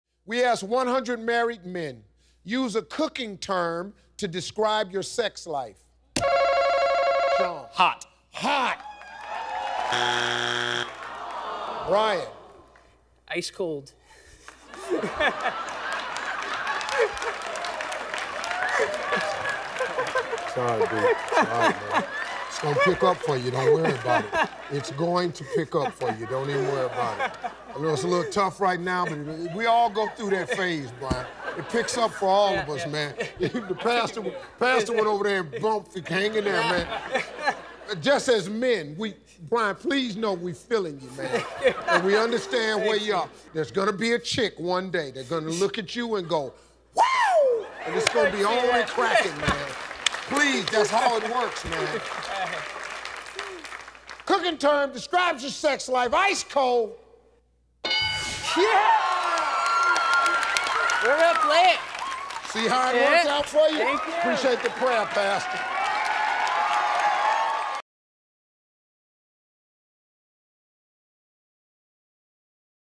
Ice Cold Love!
We asked 100 married men... use a cooking term to describe your sex life...